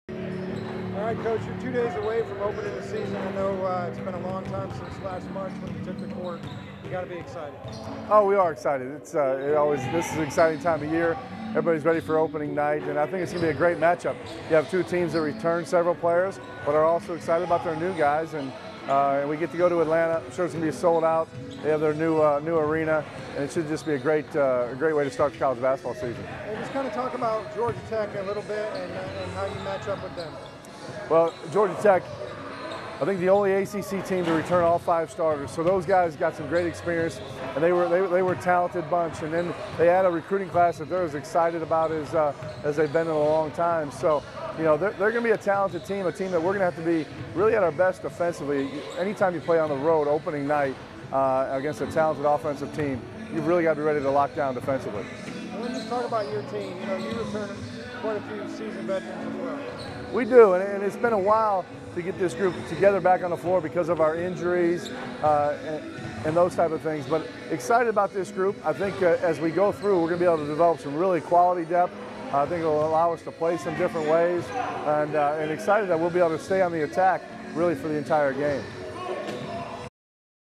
Tulane head coach Ed Conroy
Tulane head basketball coach Ed Conroy talks about the Green Wave's season opener this Friday at Georgia Tech.